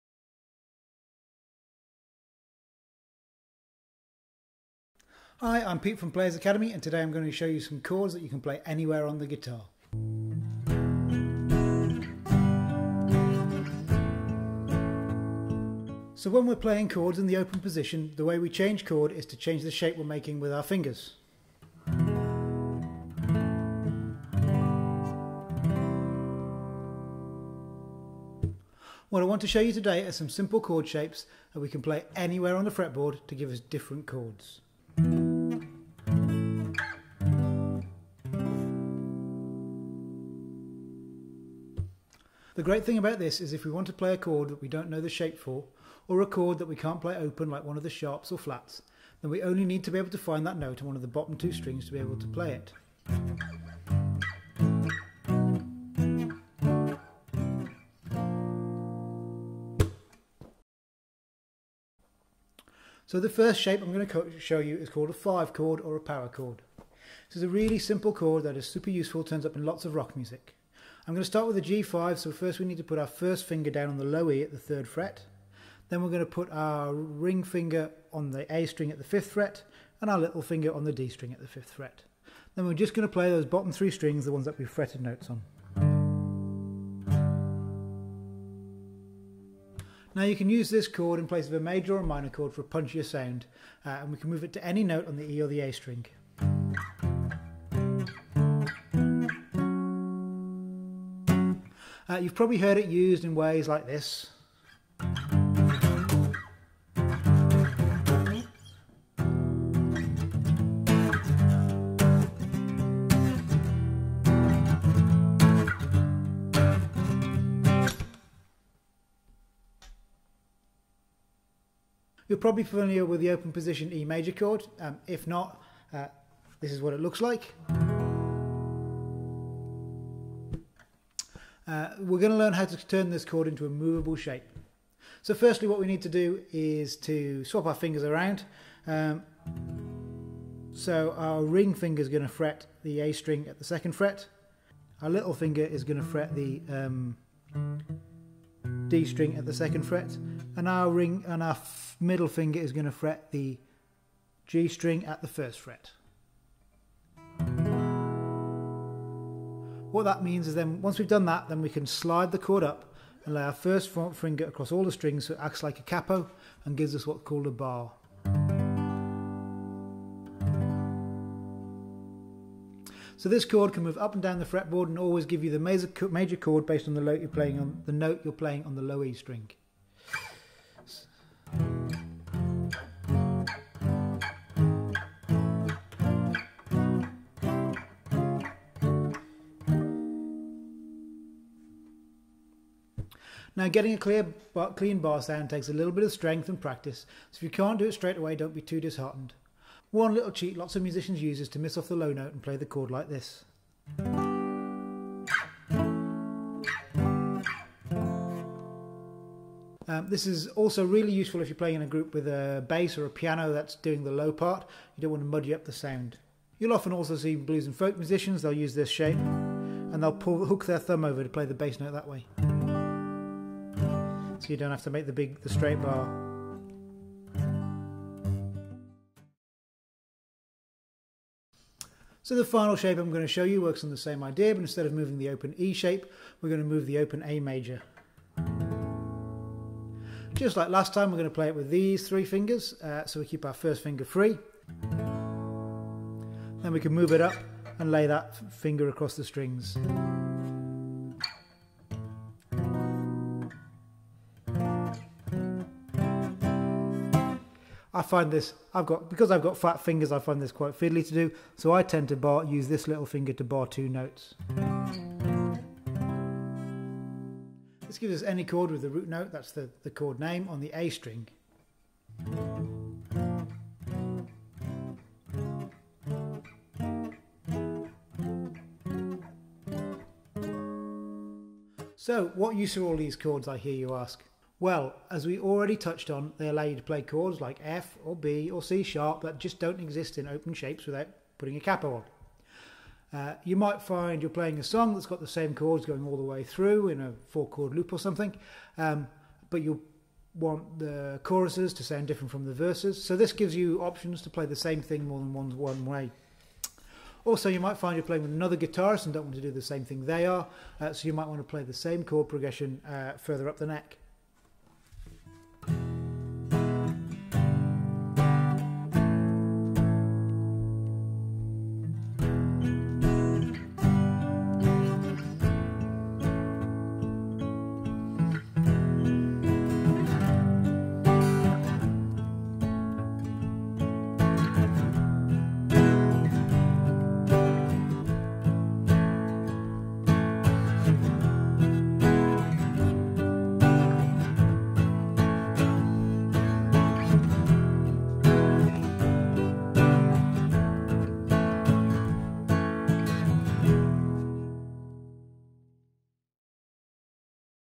Introduction to moveable chords with Pete
Something for guitarists.

Do you need some variety in your chord playing? If you're tired of playing the same open chords all the time and wish there was a way to play the same chord differently, watch on! Here, Pete shows you how to use moveable chords - same chord shape, but a whole heap of different chords. Winner!